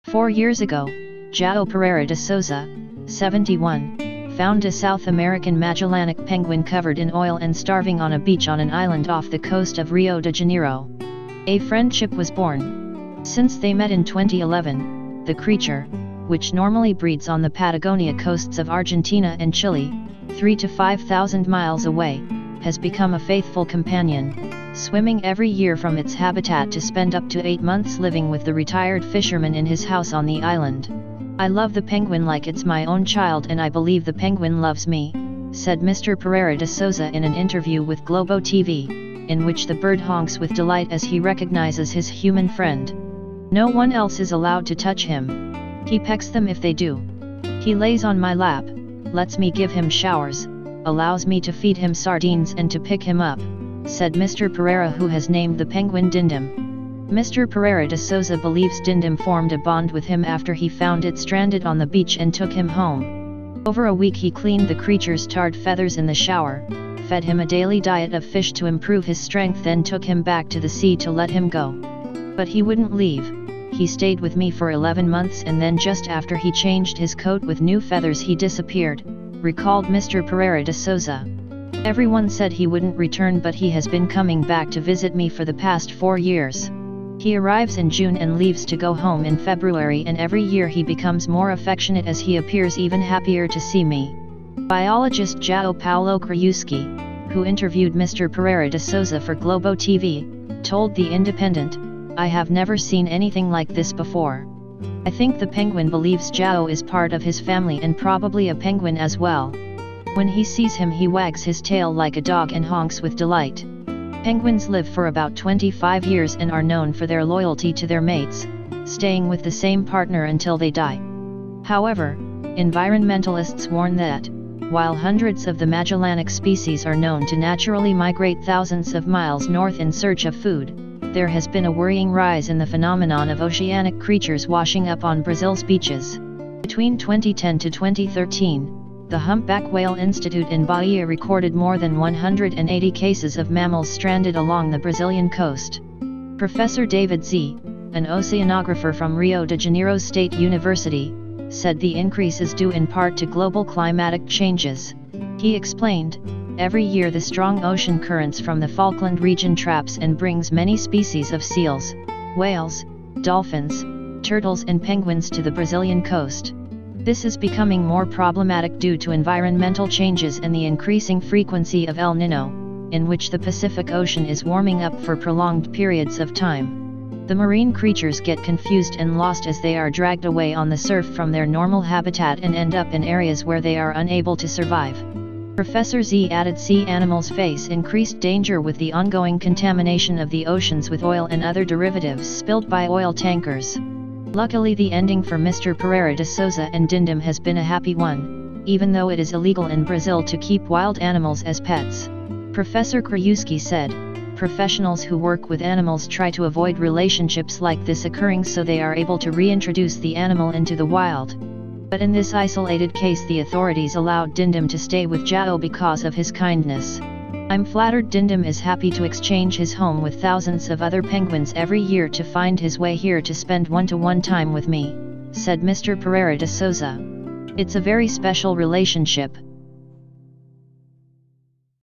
Dindim The Penguin Swims 5,000 Miles To See His Human Soulmate Every Year
Four years ago, Joao Pereira de Souza, 71, found a South American Magellanic penguin covered in oil and starving on a beach on an island off the coast of Rio de Janeiro. A friendship was born.
Since they met in 2011, the creature, which normally breeds on the Patagonia coasts of Argentina and Chile, three to five thousand miles away, has become a faithful companion, swimming every year from its habitat to spend up to eight months living with the retired fisherman in his house on the island.

Mr. Pereira de Souza believes Dindim formed a bond with him after he found it stranded on the beach and took him home. Over a week he cleaned the creature’s tarred feathers in the shower, fed him a daily diet of fish to improve his strength then took him back to the sea to let him go.

All images courtesy and credits of Joao Paulo Krajewski, Cyro Scarpa, Roberta Bonaldo, Cristiano Gomes and Globo TV.